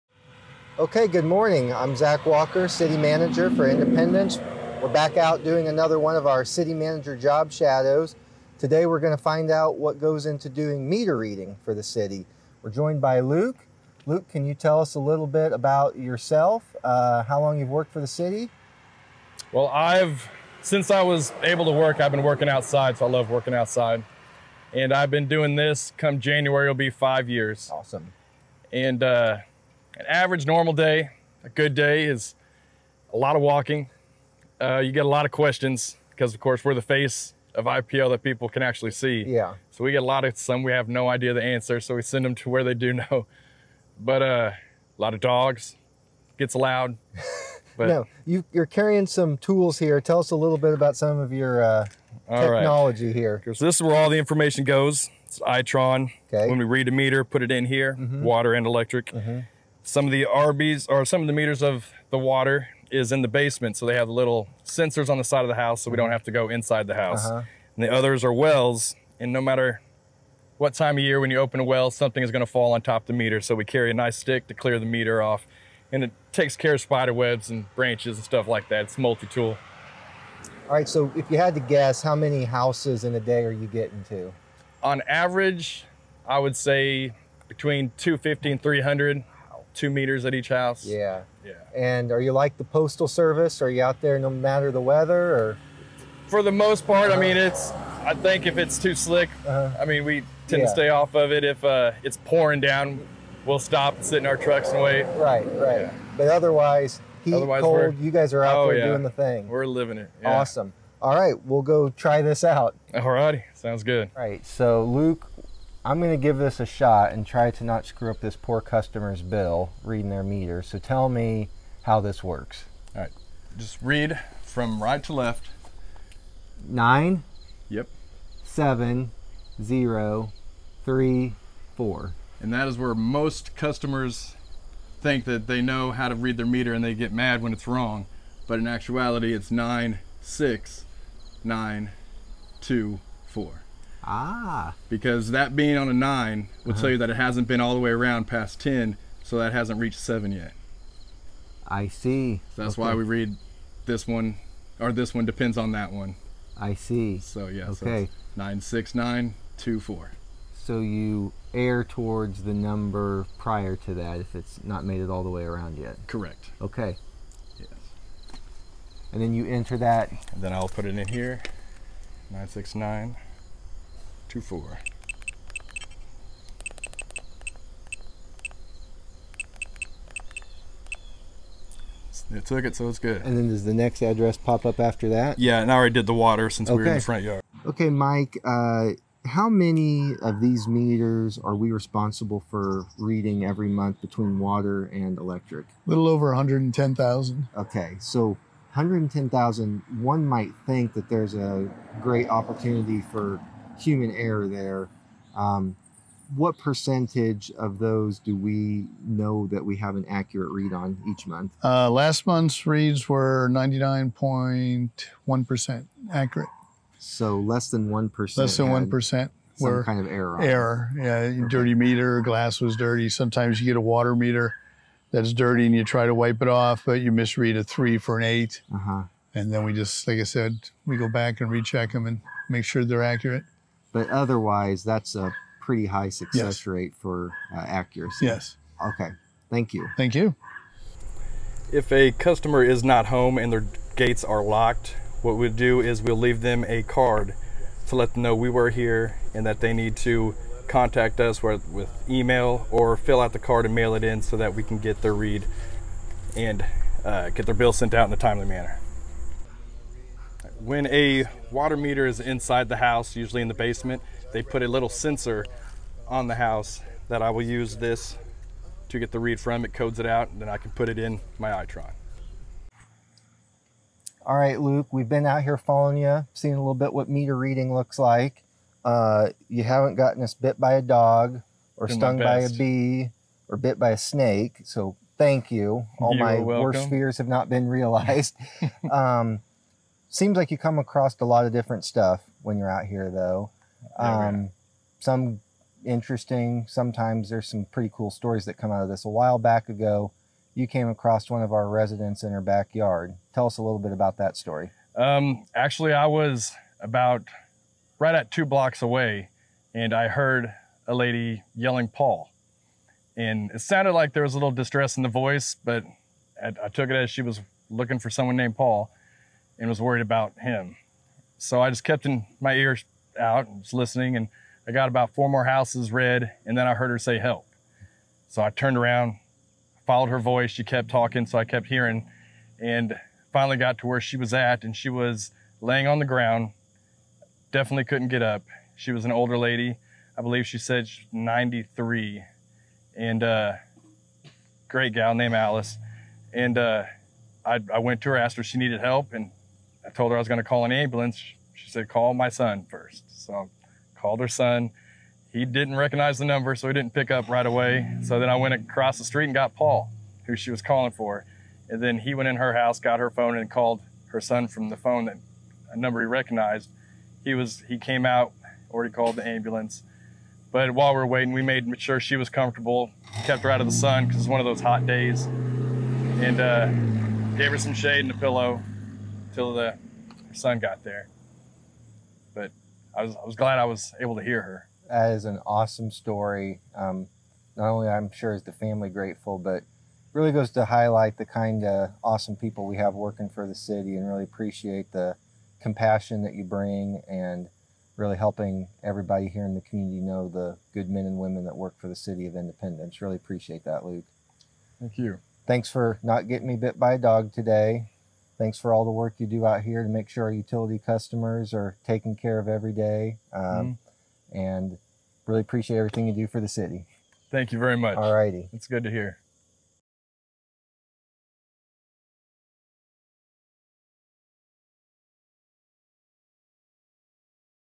Manager’s Job Shadow – Meter Reader
Independence City Manager Zach Walker spent time with Meter Reader Luke Crowder to learn more about the process. Luke also shared a special story of how he was in the right place at the right time to help one of our citizens when they needed it most.